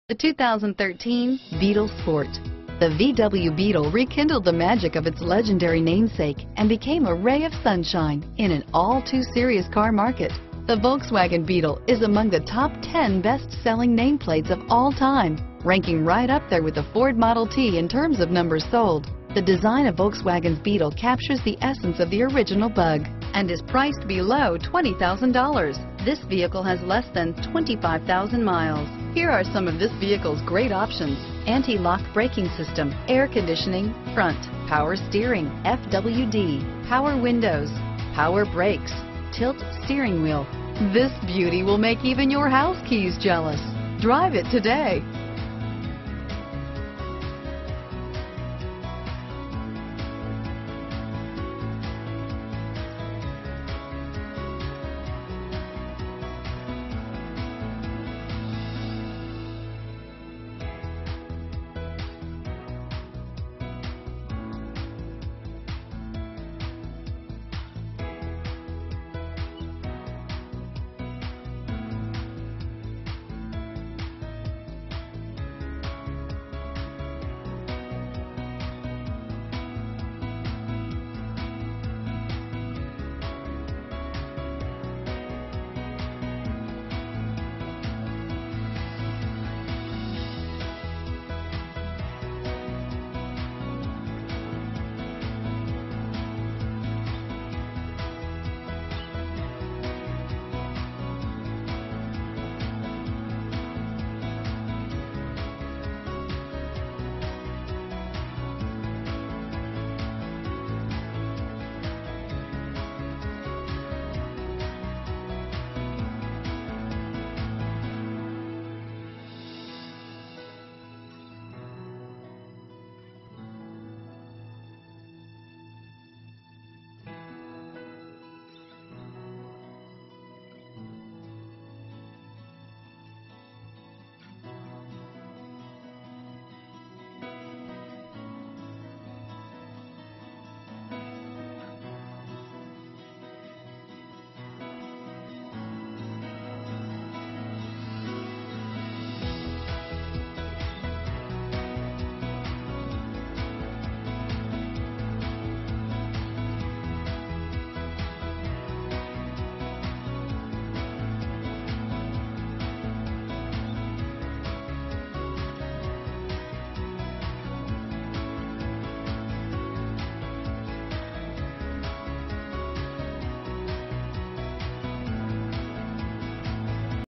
2013 Volkswagen Beetle Coupe Reno, Carson City, Northern Nevada, Elko, Fallon UC696
Nevada.
2013 Volkswagen Beetle Coupe 2.5L - Stock#: UC696 - VIN#: 3VWFP7AT3DM670598

A 2013 VOLKSWAGEN BEETLE 2 DOOR HATCHBACK WITH A 2.5L 5CYL AUTOMATIC AND FWD. EQUIPPED WITH POWER WINDOWS LOCKS AND STEERING, AM/FM STEREO WITH MP3 CD, AC, ABS, TILT WHEEL, CRUISE CONTROL AND ALLOY WHEELS... COME DRIVE THIS VW BUG HOME TODAY...